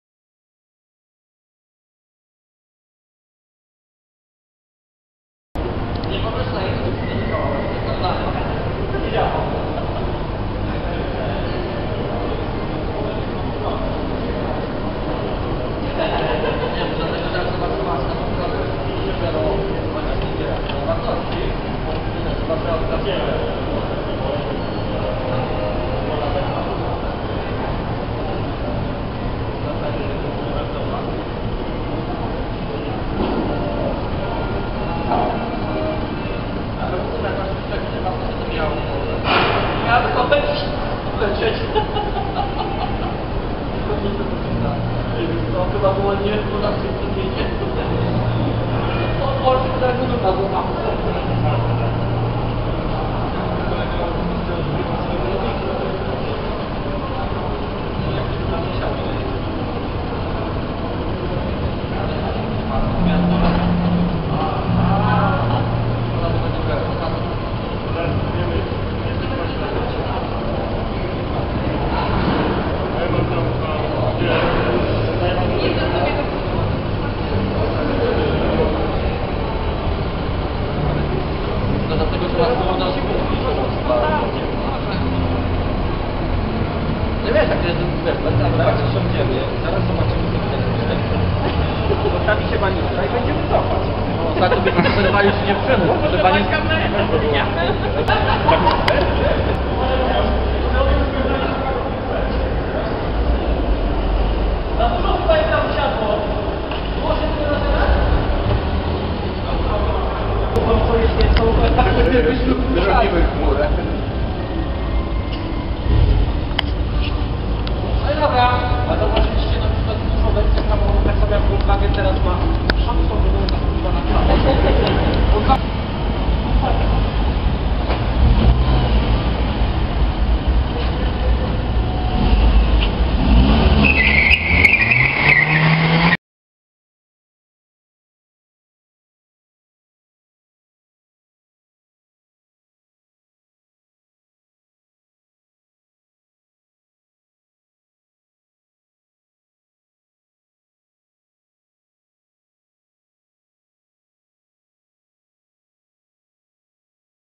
Peugeot 208 GTI
Prezentacja Peugeot 208 GTI 1,6i 16V (200 HP) na Spocie Warszawa Peugeot Klub Polska. Cena prezentowanego modelu 94 tysiące złotych / 22 300 Euro. Podziękowania la Peugeot Polska Sp. z o. o. Oddział Warszawa przy ul. Radzymińskiej 112/114 za udostępnienie modelu 208 GTI.
GTI is back :-)